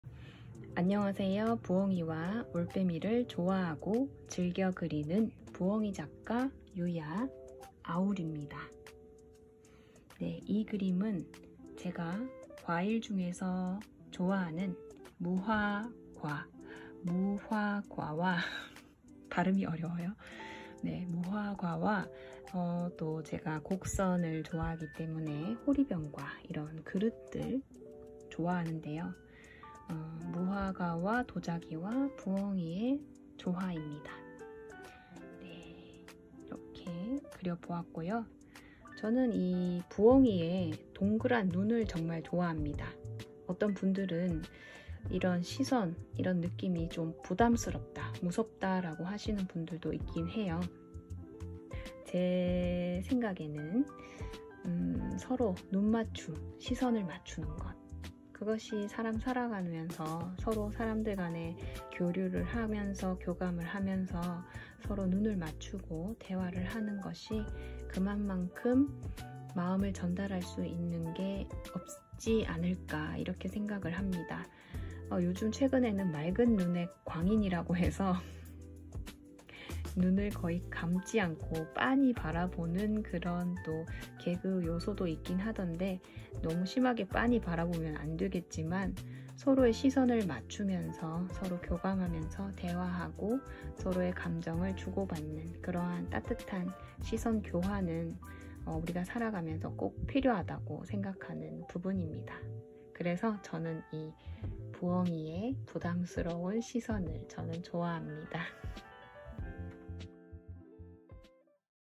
호호호 _ 무화과와 부엉이 그리고 도자기ㅣ UYAOWL Painting
안녕하세요~부엉이와 올빼미를 좋아하고
즐겨그리는 부엉이작가 유야아울입니다.

부산 전포역 6번 출구 부근에 위치한 복합문화공간에서
전시 할 그림 소개 영상입니다.